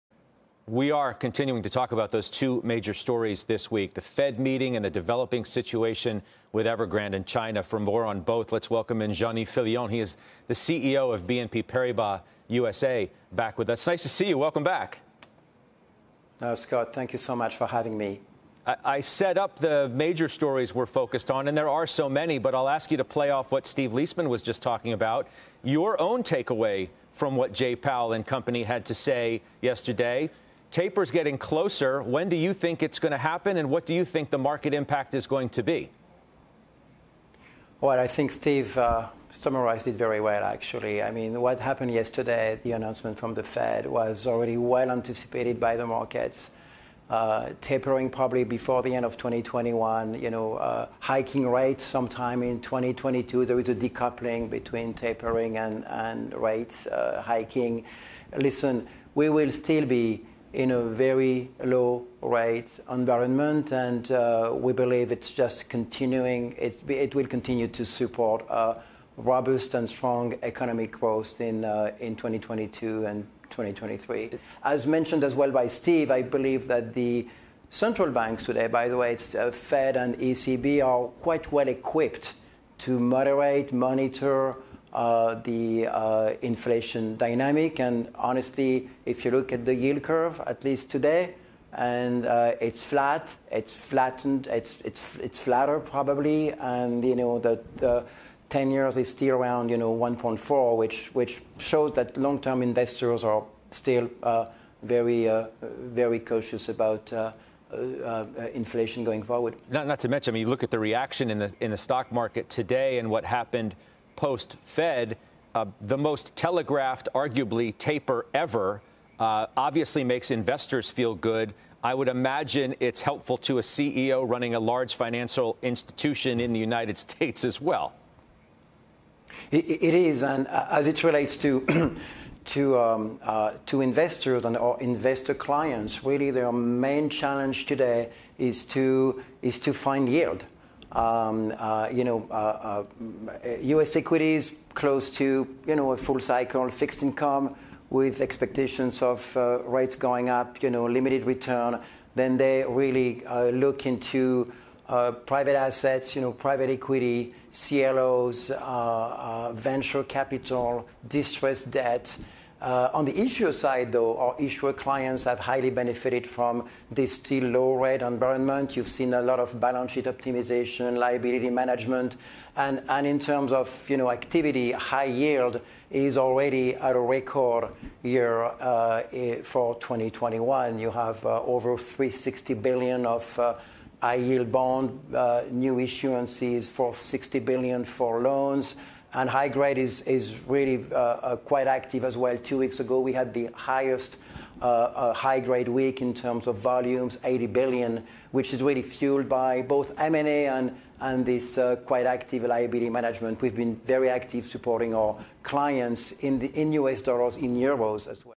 How the Fed announcement affects big banks
Jean-Yves Fillion, CEO of BNP Paribas USA, joins the Halftime Report to discuss his bank, clients and the broader market. "Our main challenge today is to find yield," Fillion tells Scott Wapner.